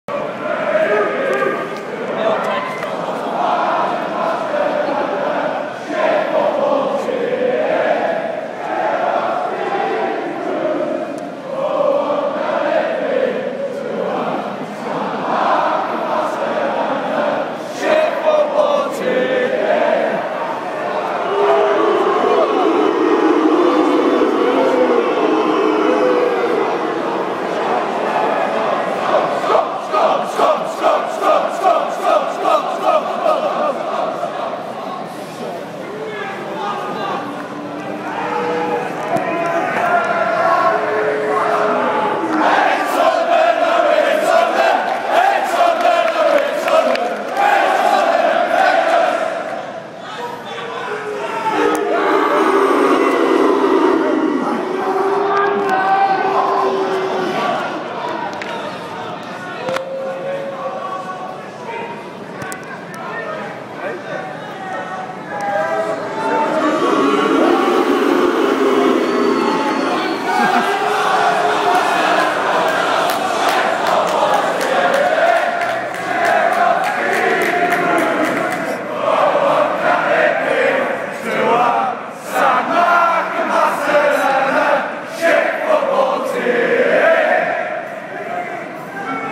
Newcastle v Sunderland Players
Sunderland players arriving at Newcastle for their 5-1 thrashing